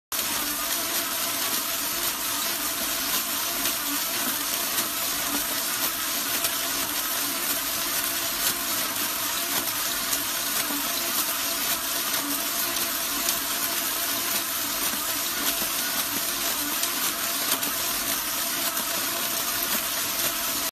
Air Cushion Pillow Bag
air cushion film/air bubbles/air pillows
1. Multi-layer extrusion film, stronger protection.
2. Pro-environment: Environmental friendly, Recyclable.
3. Easy operation: inflate on  machine, only need press start you can get what you need.
4. Save storage: inflate when you need, saving warehousing cost.
5. Different size: air bag, air tube, air bubbles, can match all your packing need.
6. Logo print available: can print your logo on the film roll.